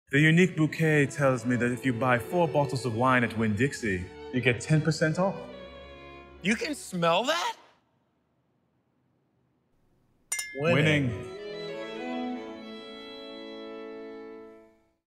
Wine Winn Win
Valid in AL, GA & FL stores only. At Winn-Dixie the WINNING never ends! Now that's a Winn Win! winnwin. Discount applies on any (4) 750ml or 1.5L bottles of wine only. Must be purchased in a single transaction, with Winn-Dixie rewards.